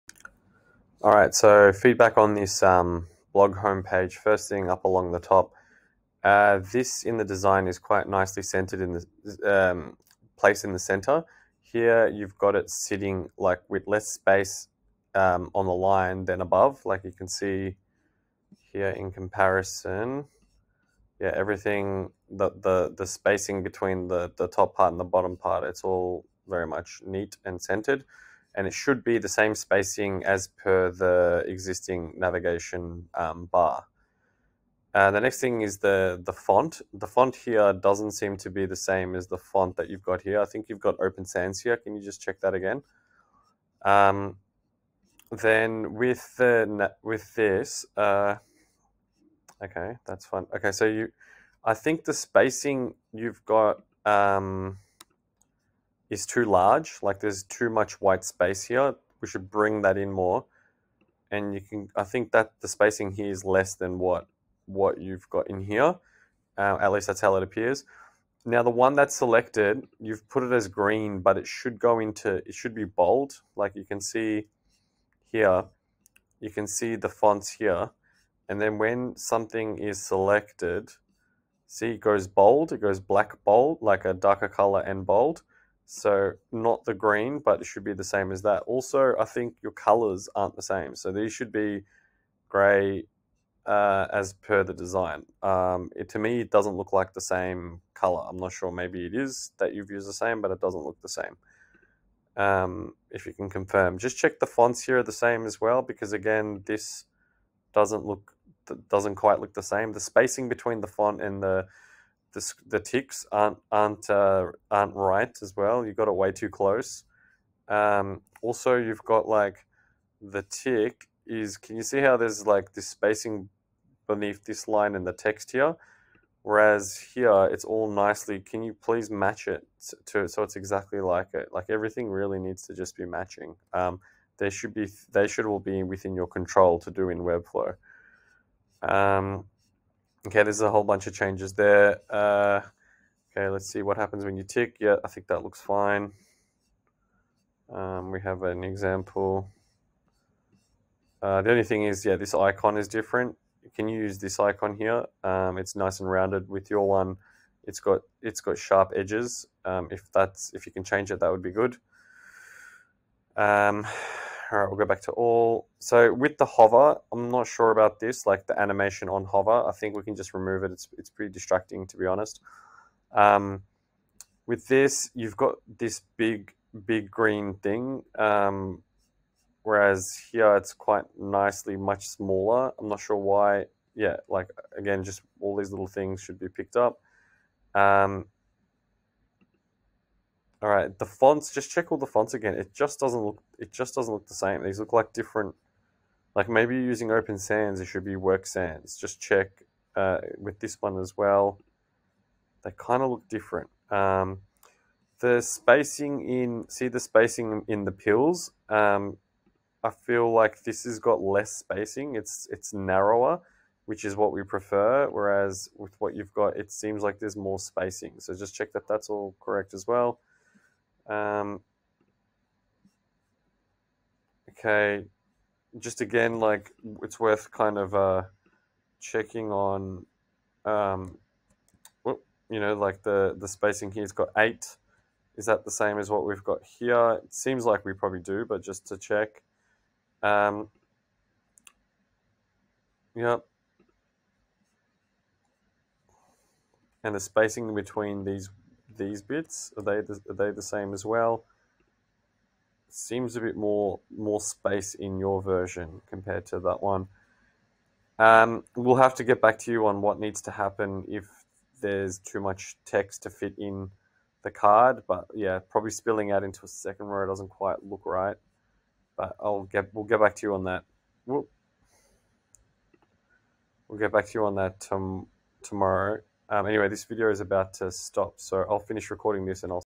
how to do QA in webflow from Figma for pixel perfect work part 1
Thanks, Onur for this video
He is one of my team members, doing QA for my developer from Figma to webflow and guiding the developer on each step. These steps will make you a perfect QA tester for pixel-perfect work without using any tool.